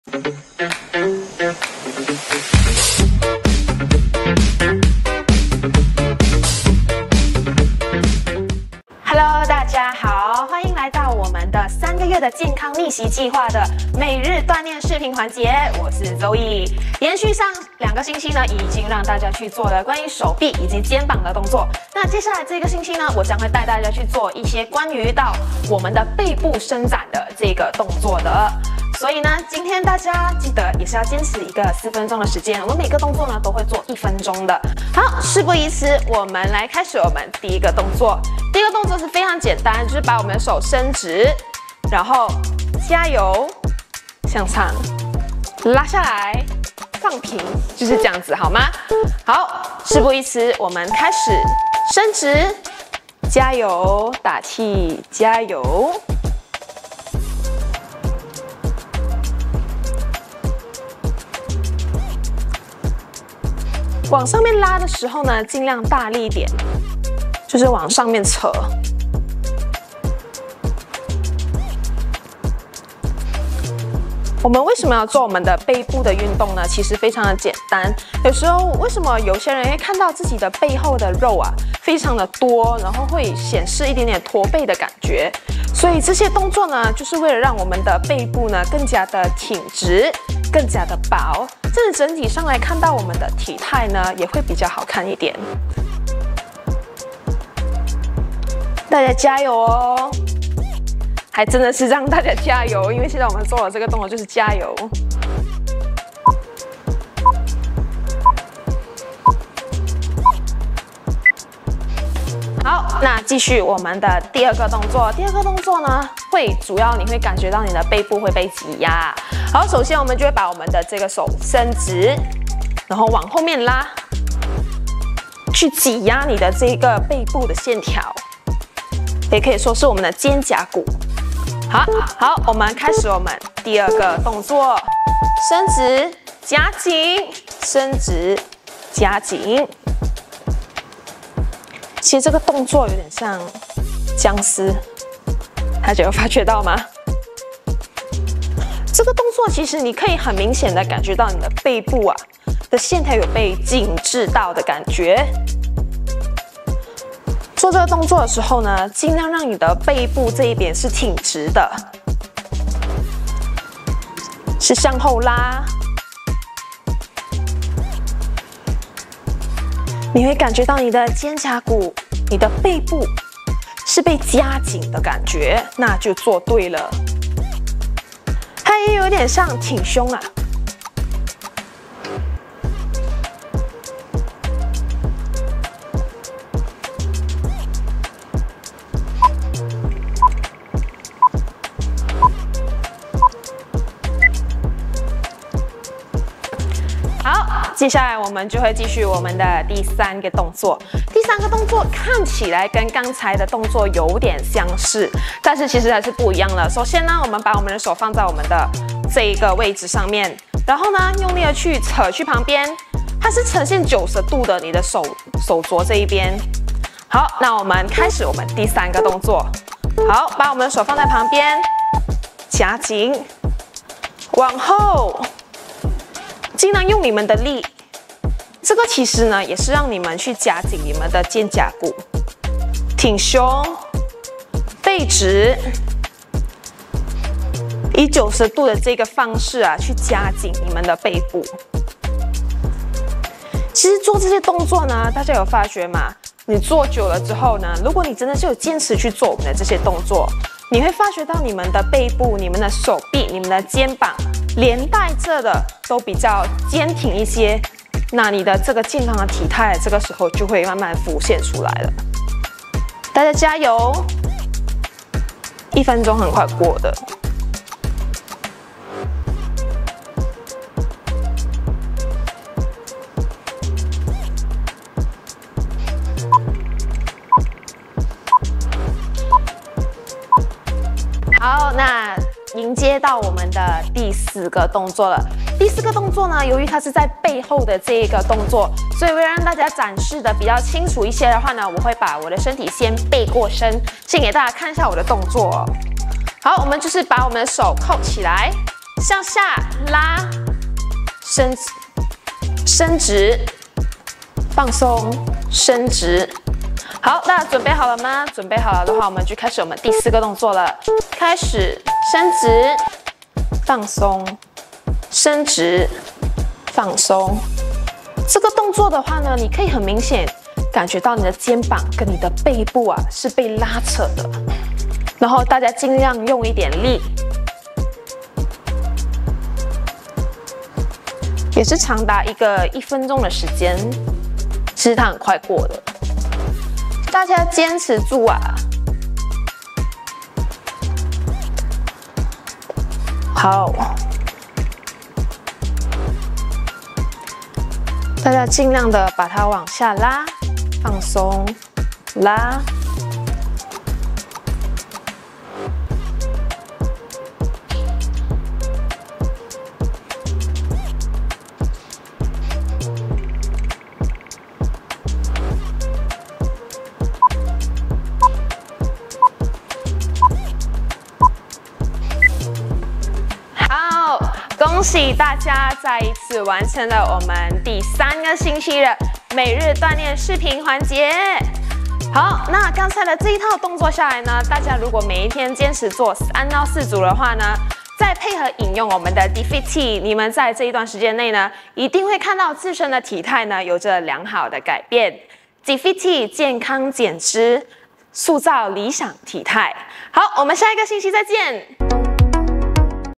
每日轻锻炼 - 第三周 3个月健康逆袭计划 Daily Workout Week 3 - 3 Month Health Transformation Program
这个星期令人振奋的每日轻锻炼来雕塑和加强您的上半身！无论您是刚刚开始想要让身体动起来还是经验丰富的健身爱好者，这个轻松简单的轻锻炼都能帮助到你实现手臂和肩膀放松，拜拜肉动起的目标，增强整体上半身力量和自信心。
Get ready for an exciting week of daily light workouts to sculpt and strengthen your upper body! Whether you're just starting and want to get your body moving or an experienced fitness enthusiast, this easy and simple light exercise can help you achieve your goals of toning your arms and shoulders, saying goodbye to flab, and boosting overall upper body strength and confidence.